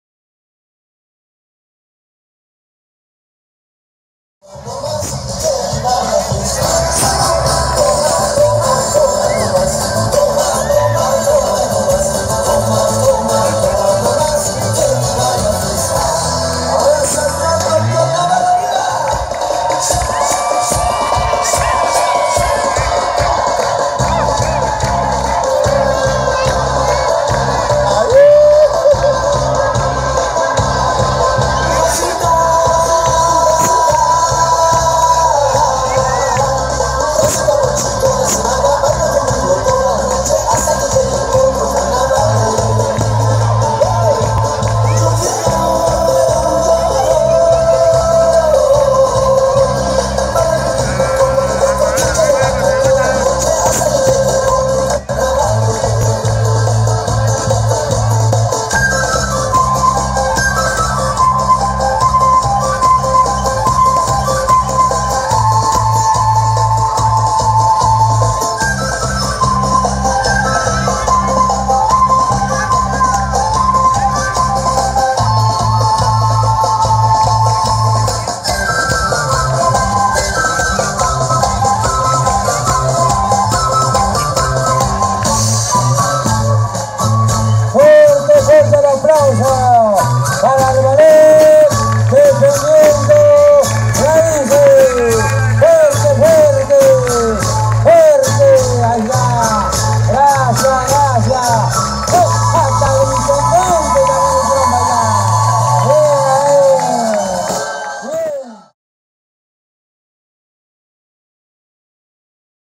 Fm Radio Speed 96.7 Mhz - 2da.Noche - Cantando Bajo los Cielos de Fraile
2da Noche, organizado por la Municipalidad de Fraile Pintado - Cantando Bajo Los Cielos de Fraile. Esta oportunidad, bailando Iván Euclídes Poncio, el Carnavalito Jujeño y el Pim Pim de nuestra zona.